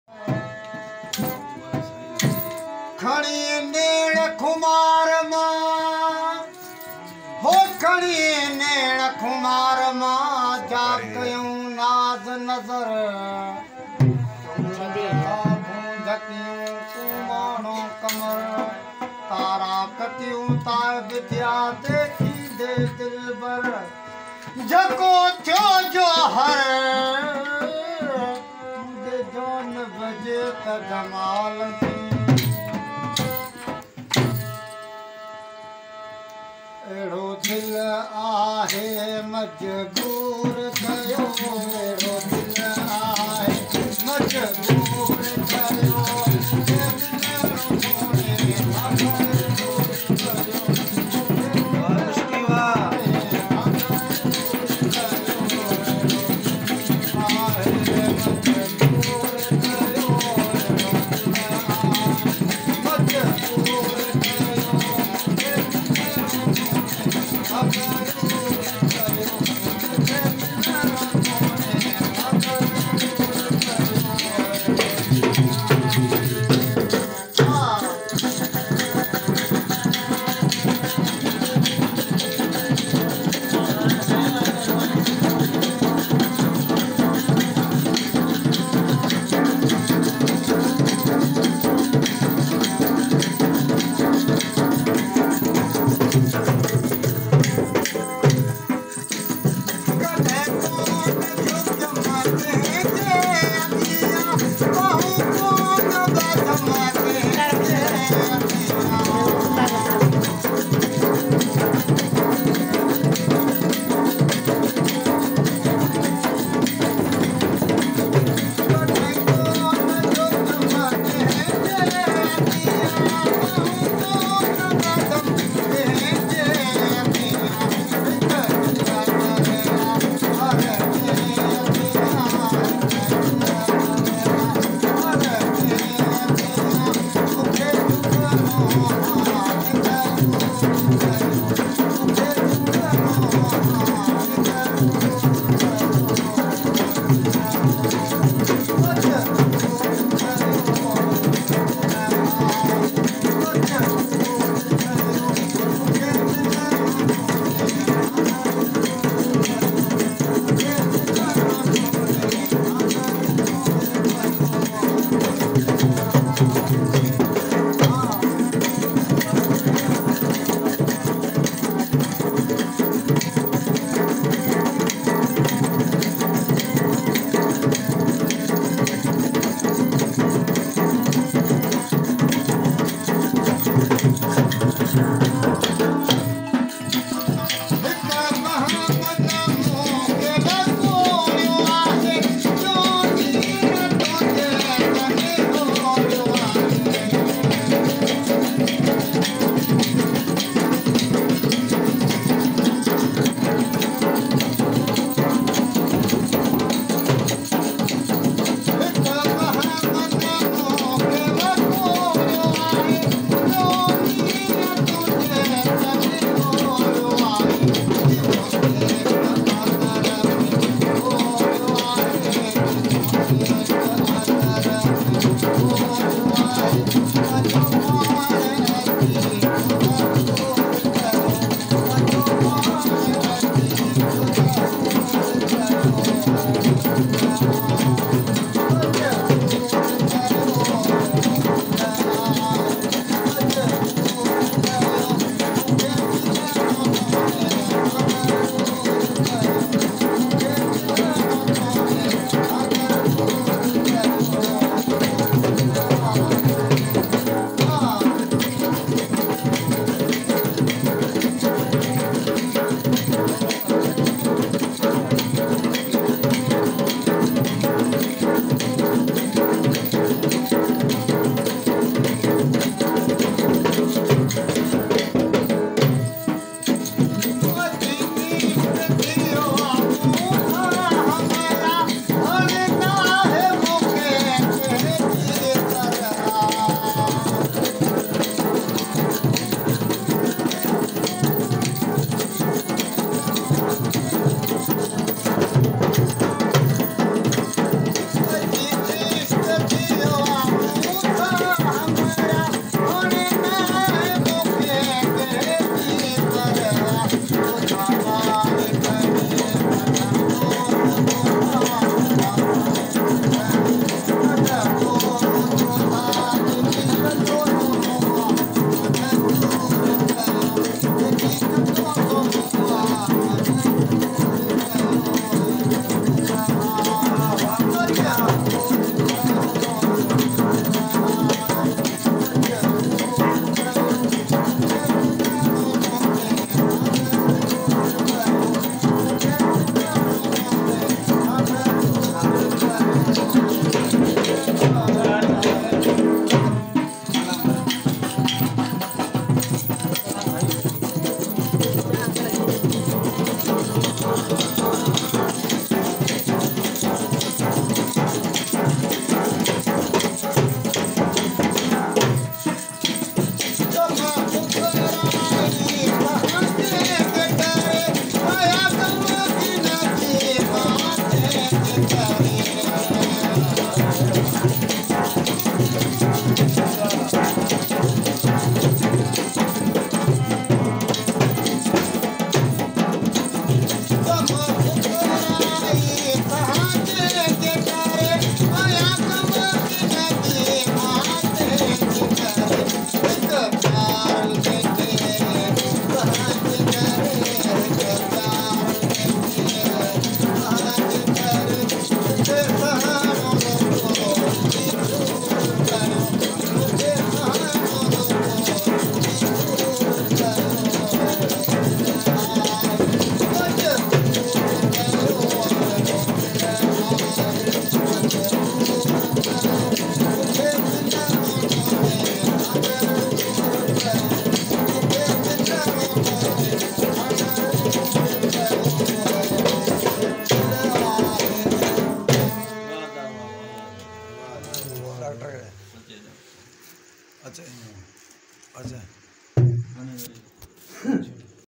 Sofi Song (Village Jaffar Khan Laghari)
Sofi Mehfil m ustad Jabbar Solangi pahnjhi mehfil pesh kry rha ahen (Foqurai)